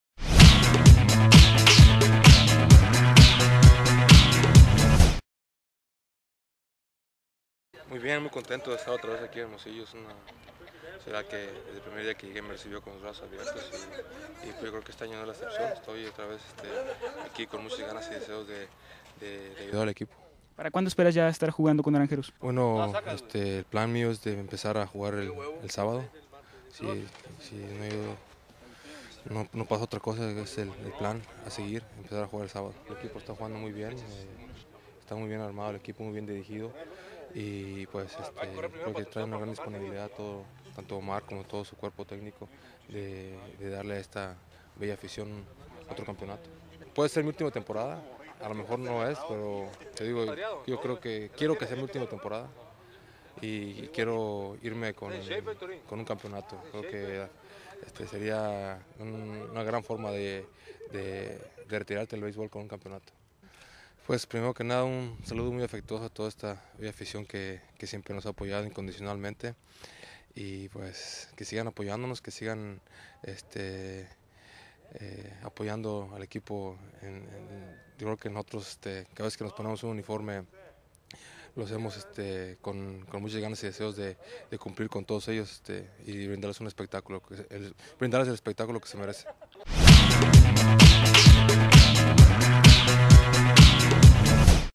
Vinicio Castilla reporto con Naranjeros
Vinicio Castilla, reportó con naranjeros de Hermosillo para lo que podría ser su última temporada según afirma el mismo ligamayorista.